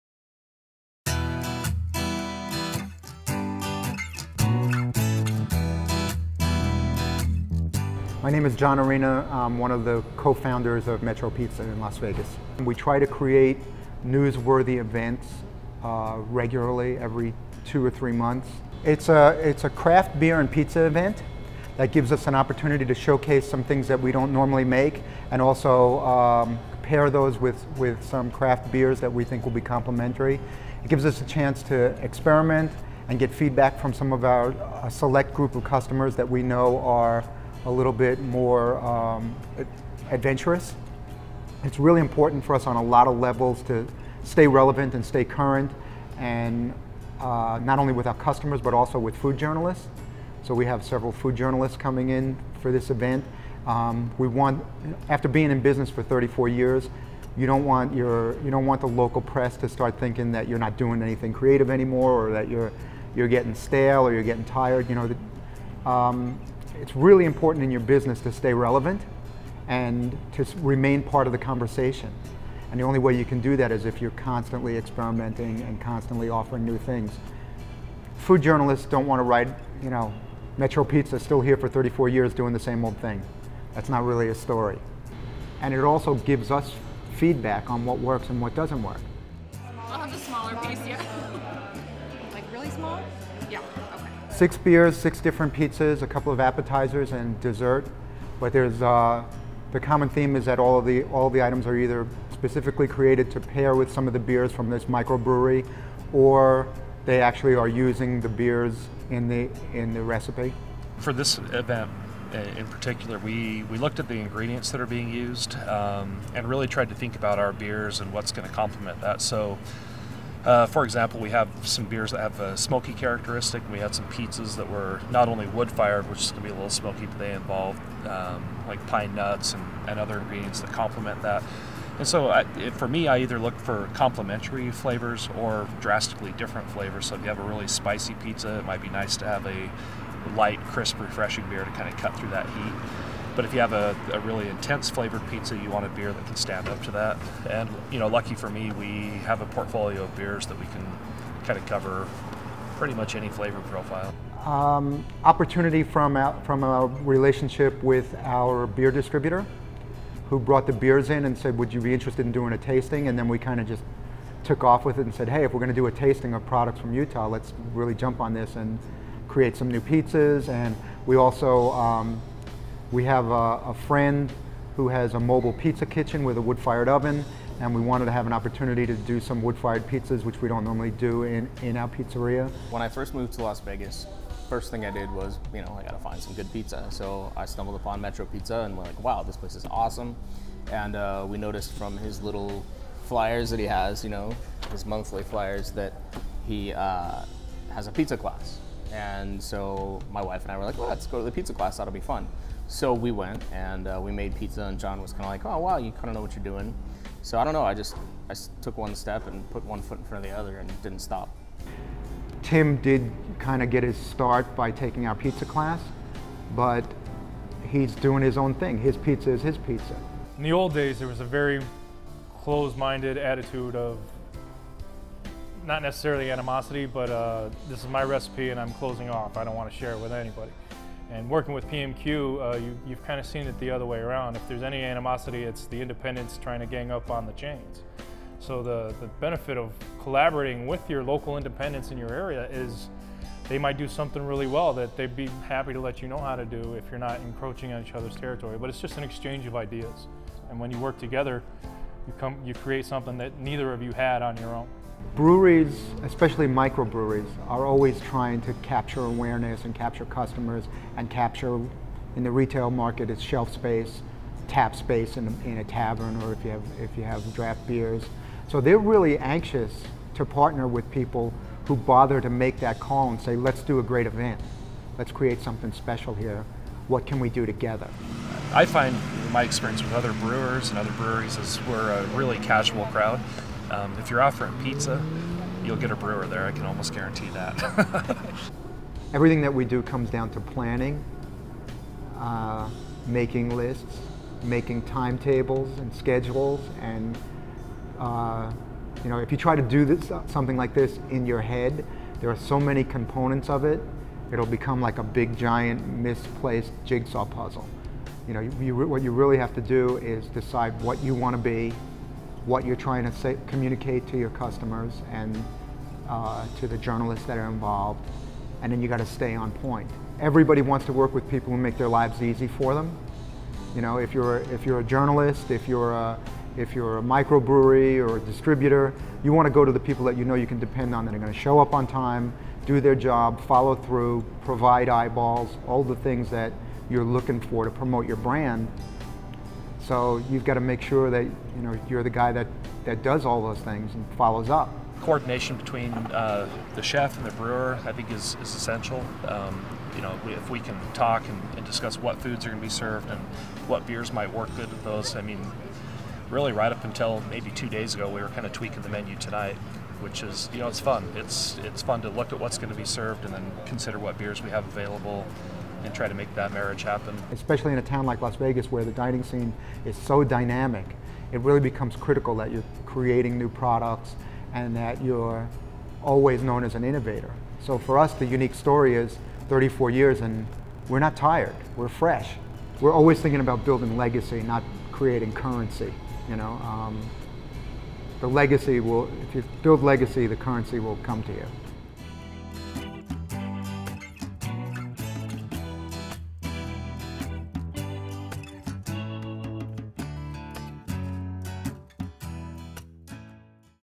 How to create a beer and pizza pairing event
John Arena of Metro Pizza, Tim Younghans of Mama Napoli Pizza and the folks from Wasatch Brewery share how to create a beer and pizza pairing event.